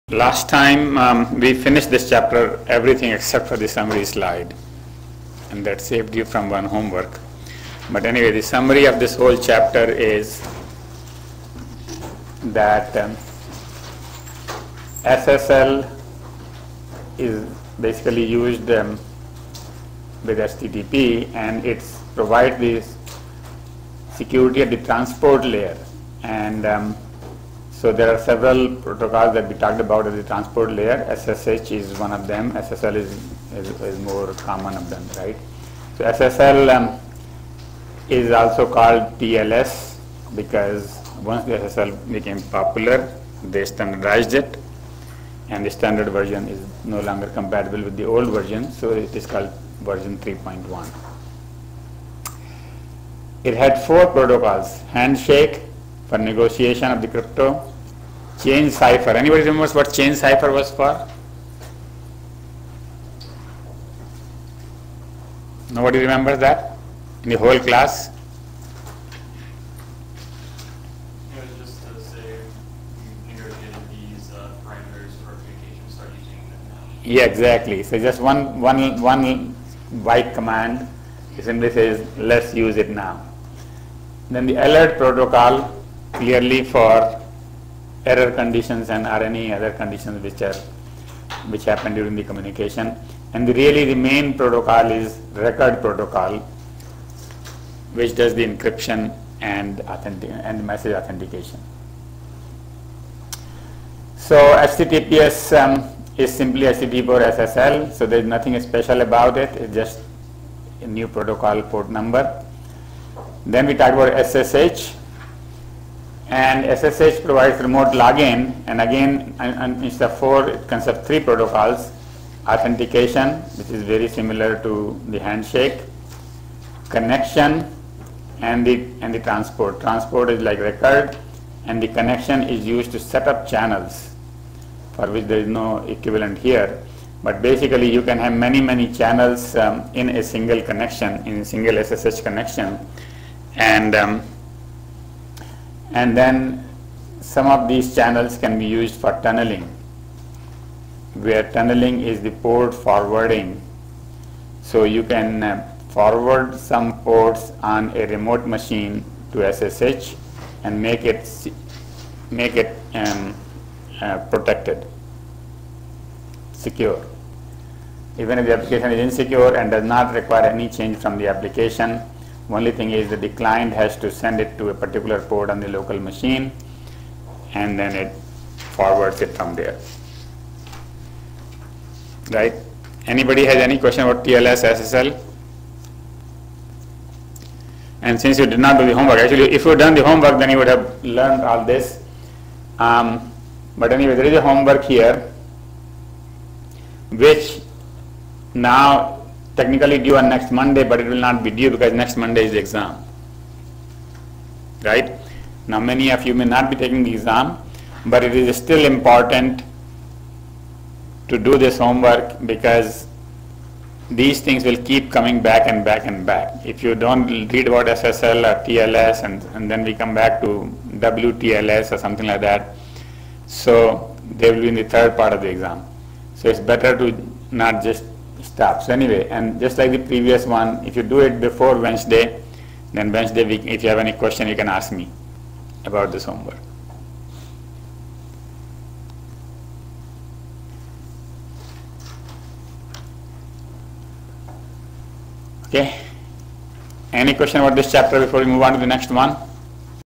CSE571-11-16B: Transport Level Security (Part 2 of 2)
Second part of Audio/Video Recording of Professor Raj Jain's class lecture on Transport Level Security.